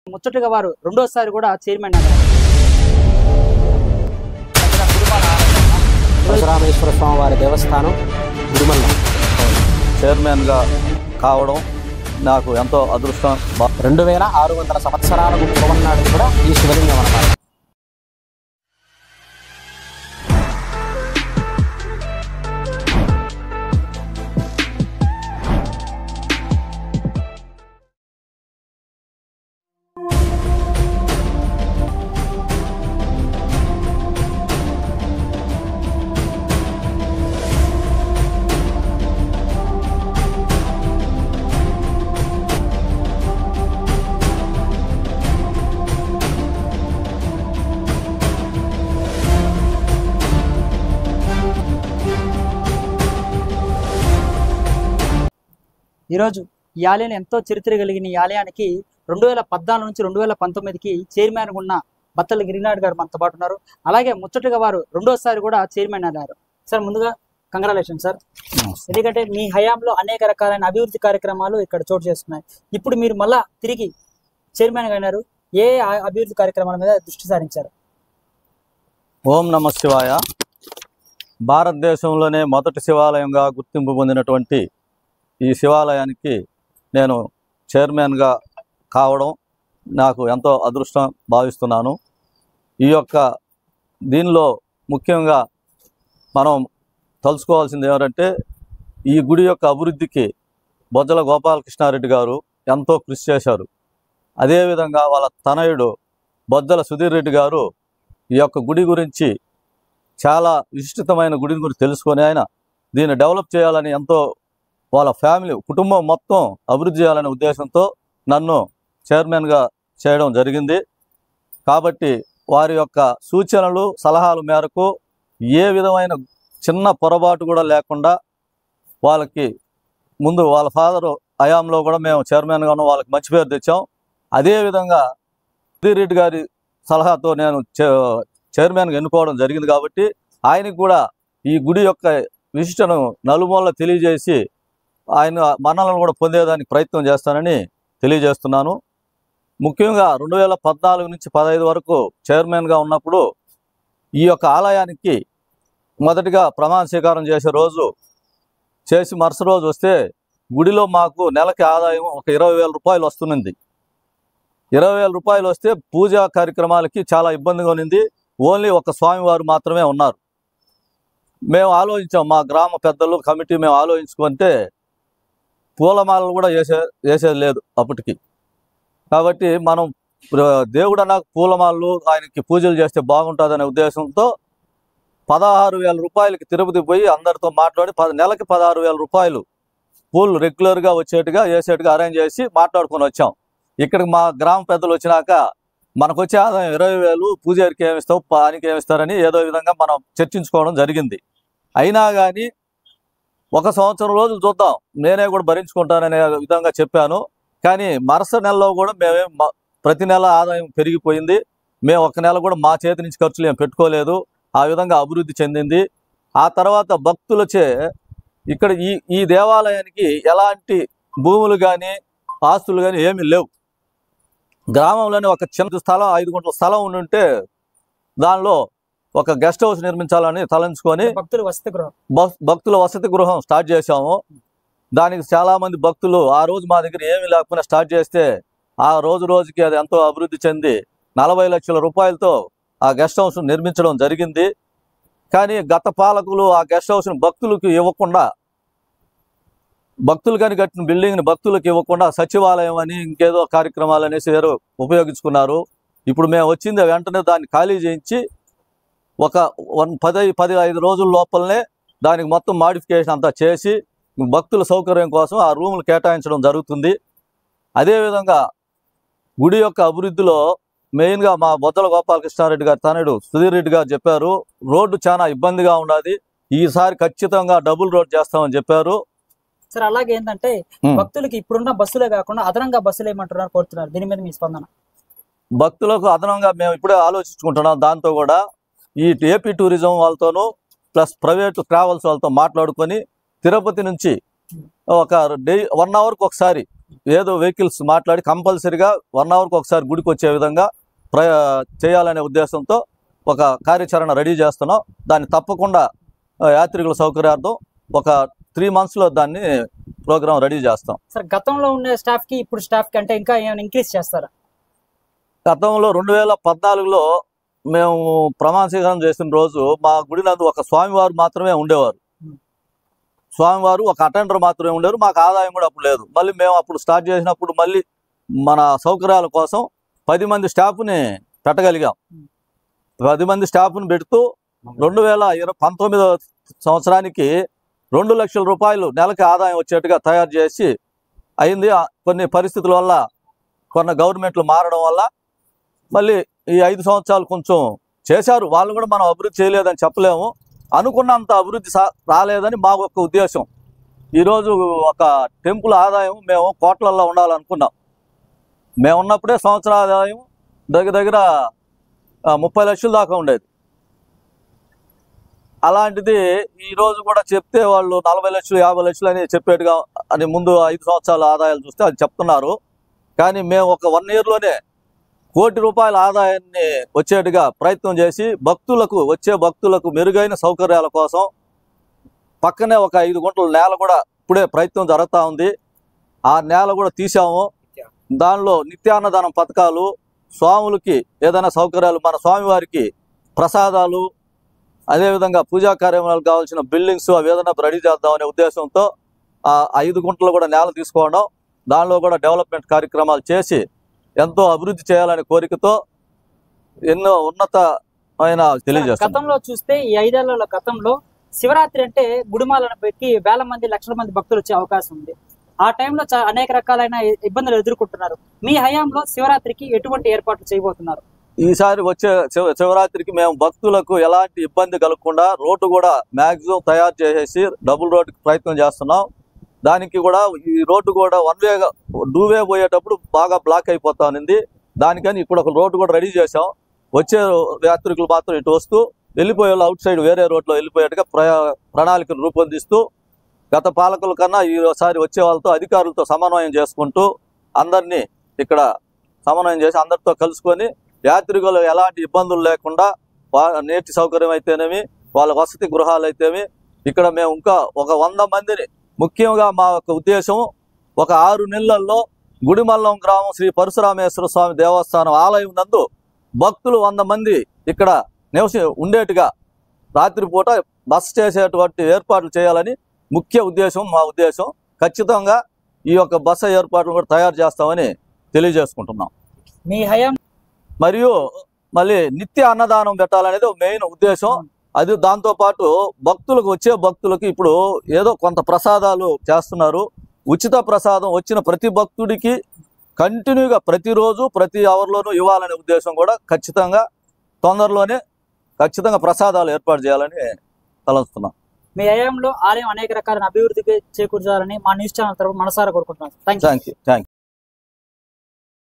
గుడిమల్లం ఆలయం నూతన చైర్మన్ గిరి నాయుడుతో ముఖాముఖి || dhanush royal gudimallam temple new chairman
gudimallam temple